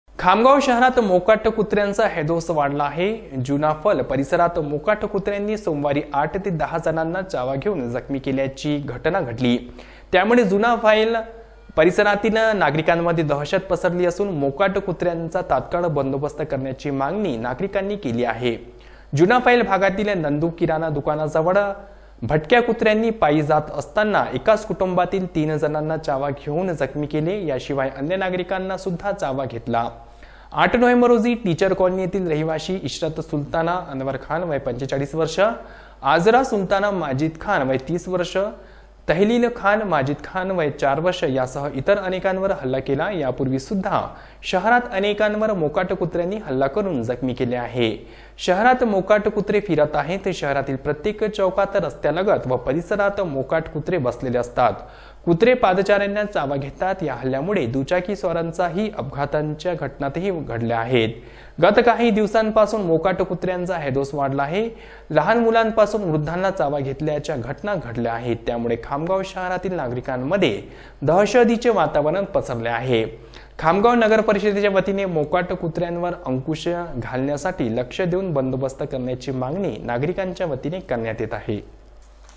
RN NEWS खामगाव येथे मोकाट कुत्र्यांचा हैदोस दहा जणांना चावला
खामगाव येथे मोकाट कुत्र्यांचा हैदोस दहा जणांना चावला